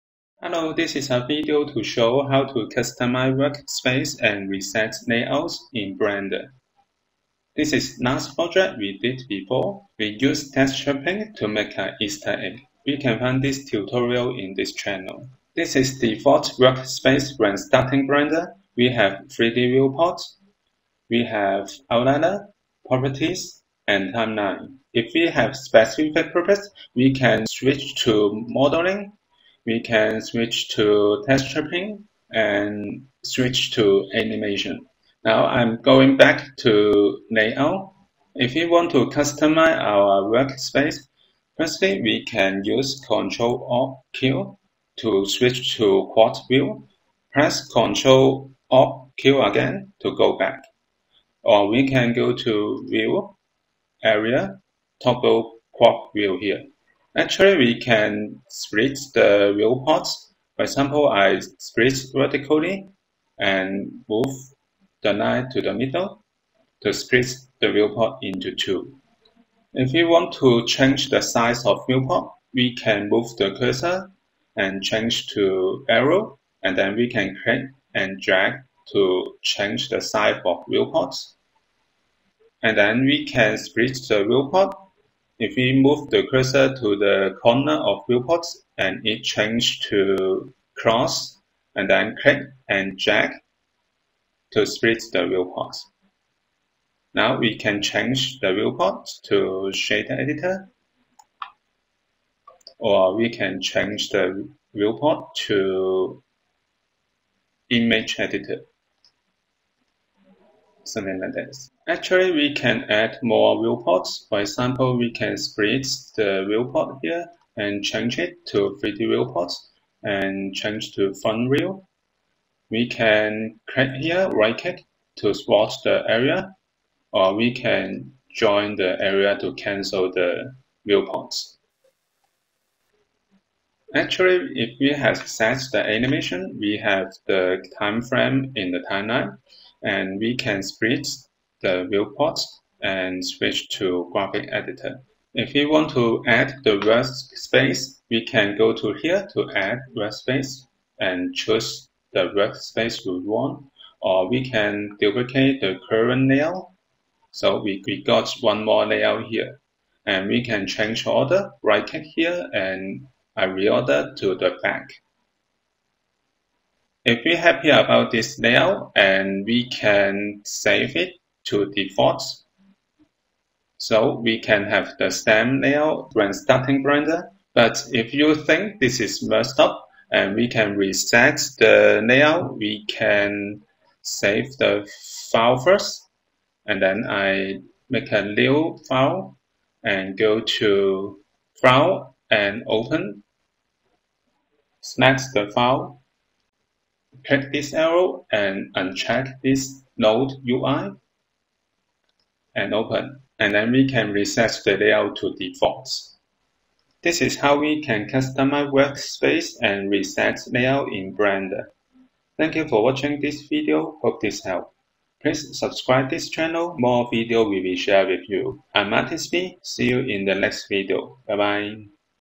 Blender Reset Workspace to default, Add and Close viewport window for layout
Tutorial video to show how to reset workspace to default and add & close viewport window for layout. In this Blender Layout Tutorial for beginners, we start from Blender default workspace and layout. Then we can add workspace window, split viewport, resize the layout, swap the areas and join the areas. Also, we can add and duplicate the workspace ,and save workspace layout as Blender default. But if the workspace layout was messed up, we can reset layout. Steps in this tutorial are easy to follow, hope this helps.

Timestamp
0:00 Blender Layout Tutorial
0:15 Default Workspace in Blender
0:41 Add Workspace Viewport Window in Layout
1:01 Split Viewport
1:15 Resize Layout
1:38 Change Editor Type
1:54 Add, Swap, Join areas
2:29 Add and Duplicate Workspace
2:49 Blender save workspace layout as default
3:02 Reset Workspace Layout in Blender